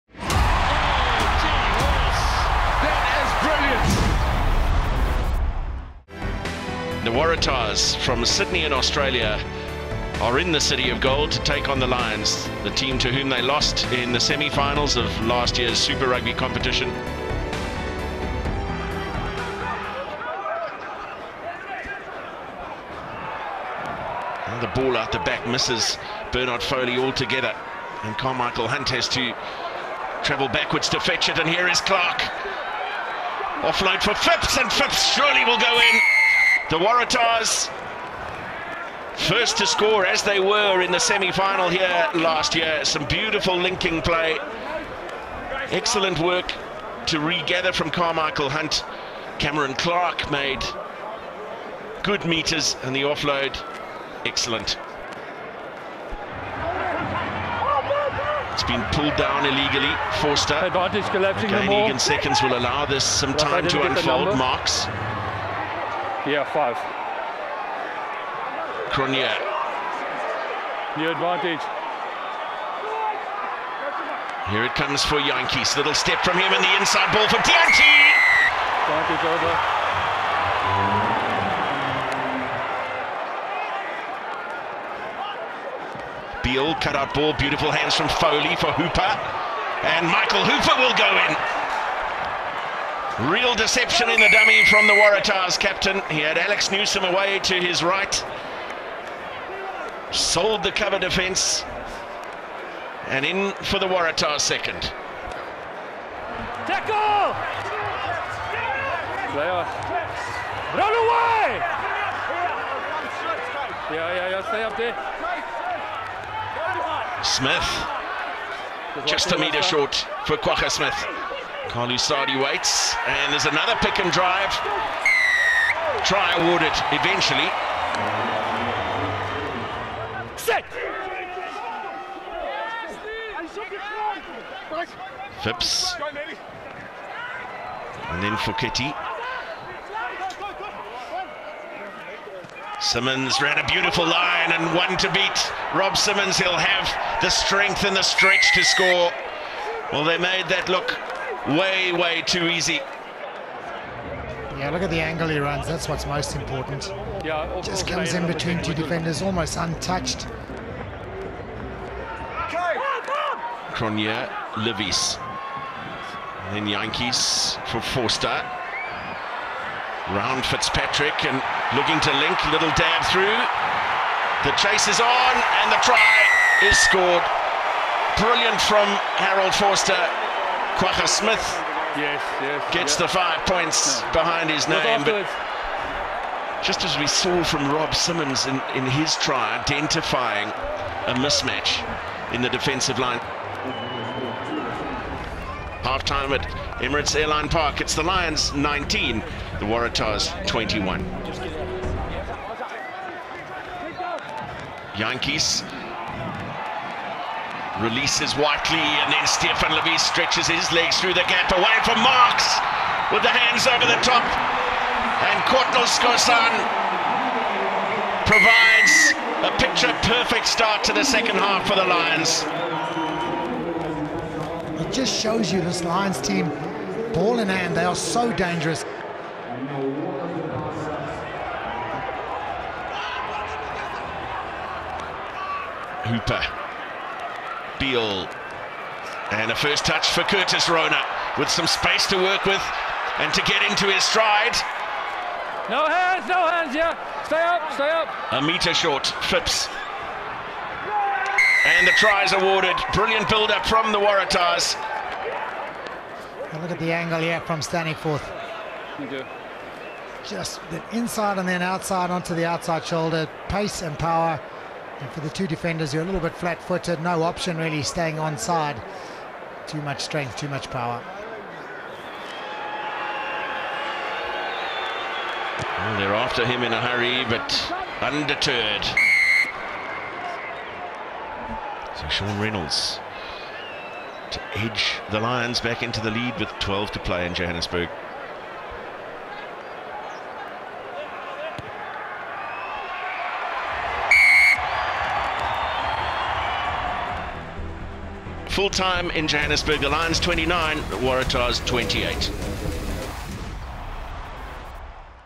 Lions v Waratahs | Super Rugby 2019 Rd 13 Highlights
A see-sawing encounter in Johannesburg has seen the Lions prevail 29-28 over the Waratahs.